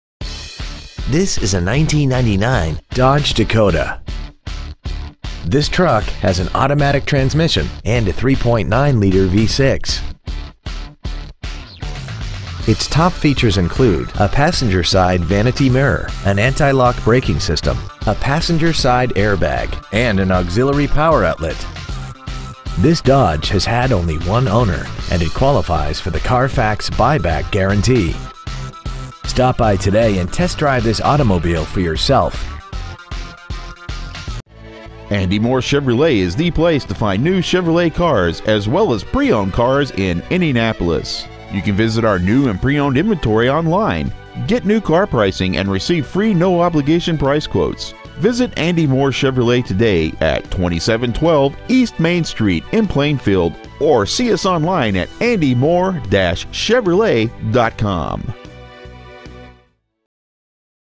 1999 Dodge Dakota Indianapolis IN 46168
Year : 1999
 Make : Dodge
 Model : Dakota
 Engine : 3.9L V6
 Trans . : AUTO
 Exterior : Bright White
 Miles : 225,324
 Interior : Mist Gray
 Stock : PT1908B

 2712 E. Main Street